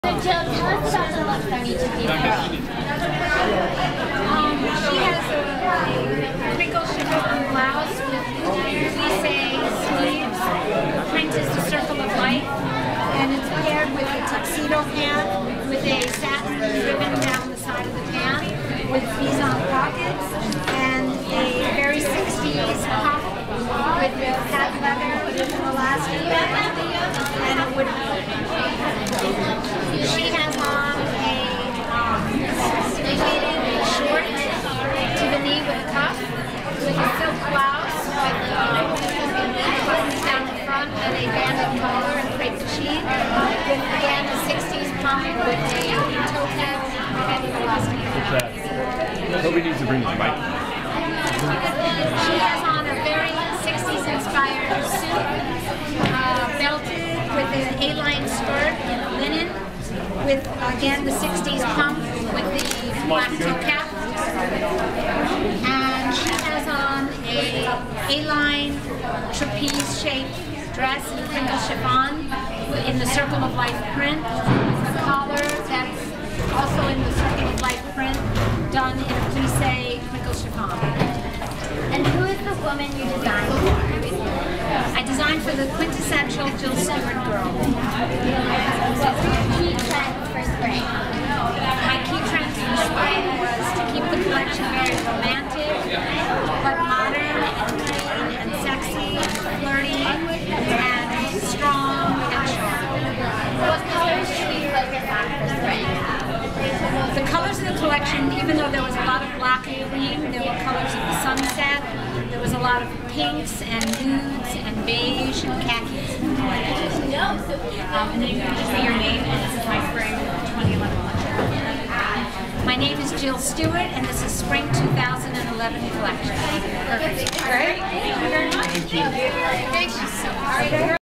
Designer Q&A with Jill Stuart
Designer Jill Stuart talks about the inspiration for her Spring Summer 2011 collection backstage following the runway show during Mercedes-Benz Fashion Week in New York City. Video by The Budget Babe